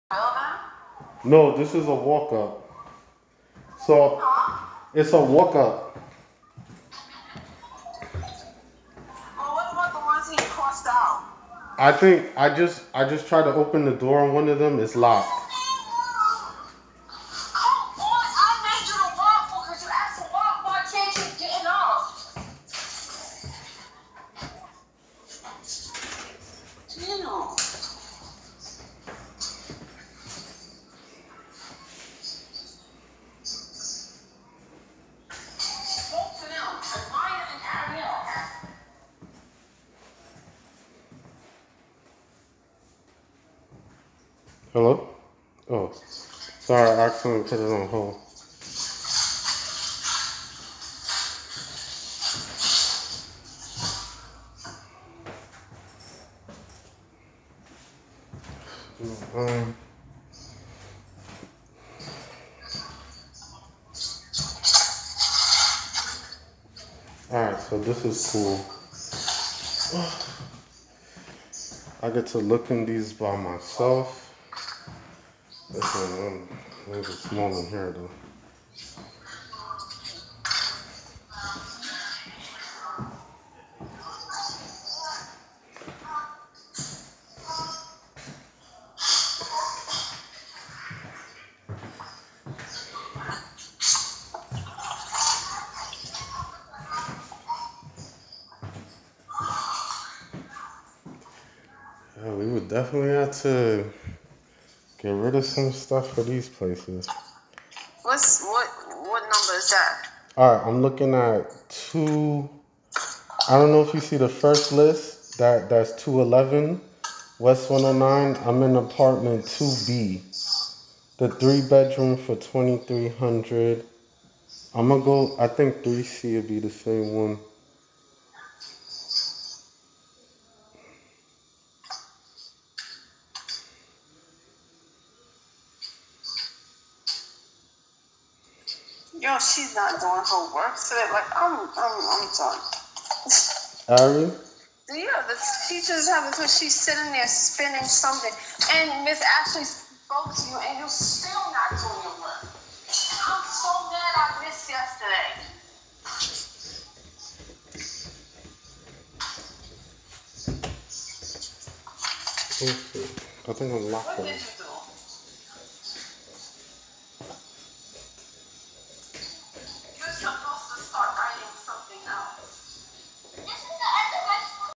The Apartment Hunt NYC Series Episode 11
The Apartment Hunt NYC Series - my quest to find a decent apartment during the pandemic